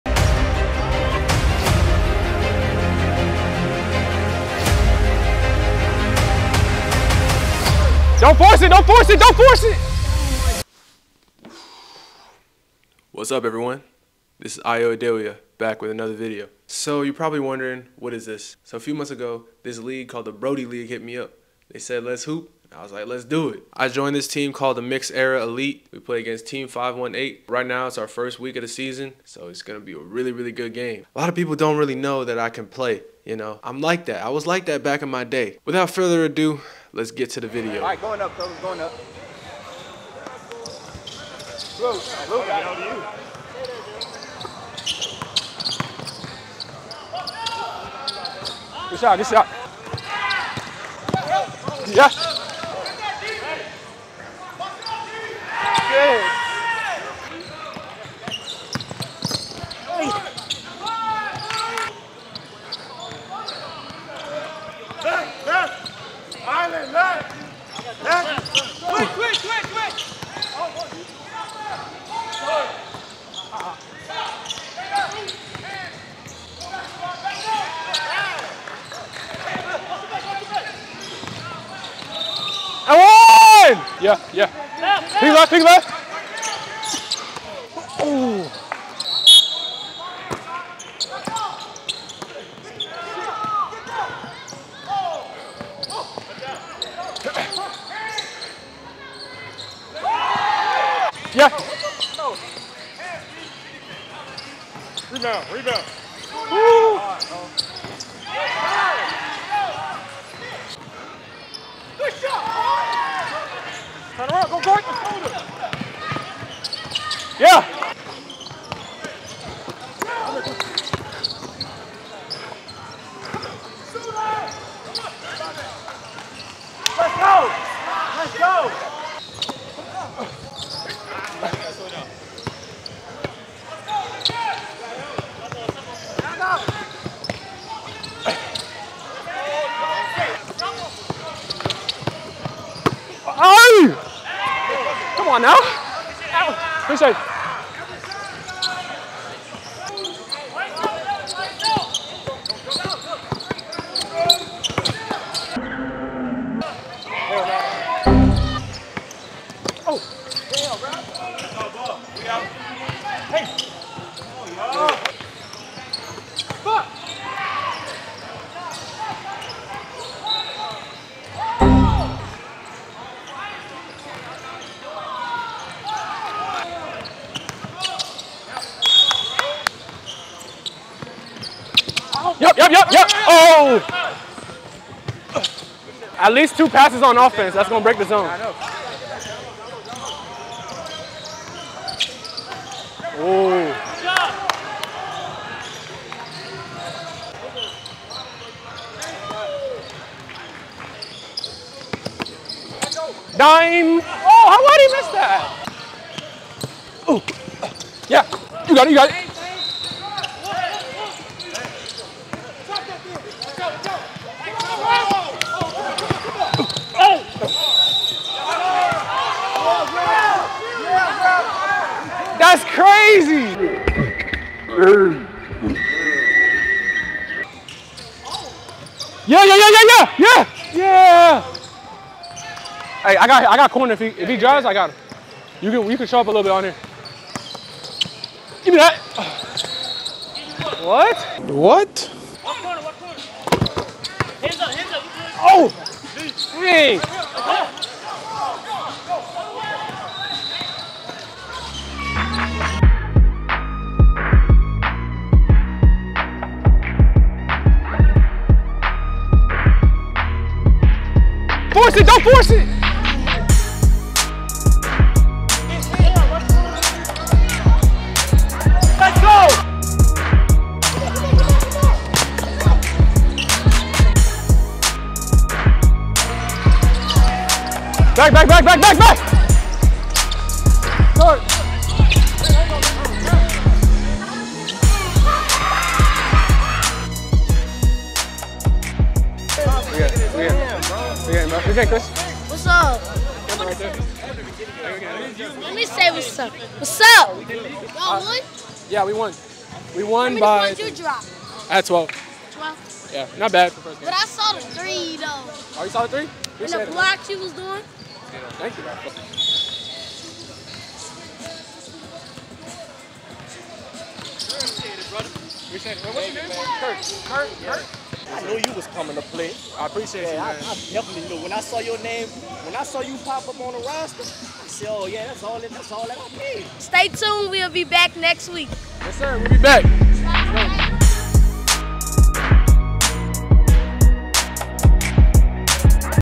BRODIE LEAGUE EP 1--KIRT GOES KRAZY
Welcome to my new series of the Brodie League! The Brodie League is one of the best men's leagues in North America. I'll playing in the Fall Season and taking you behind the scenes.